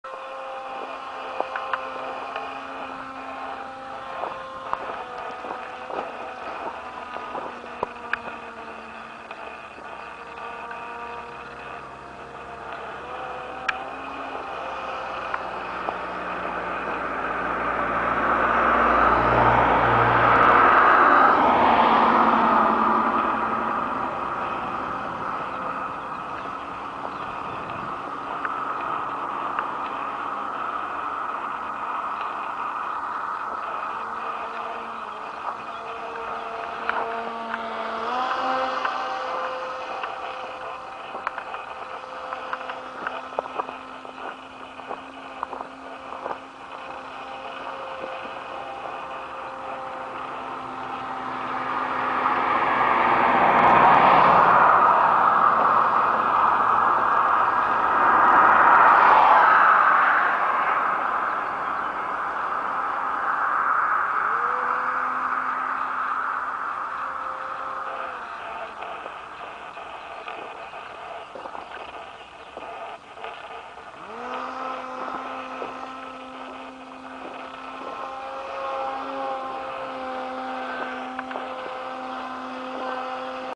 ma deuxieme bionic rc
voici un petit paramoteur rc de deux metre d envergure,stable se pilote bien,modele a vendre pour les interressé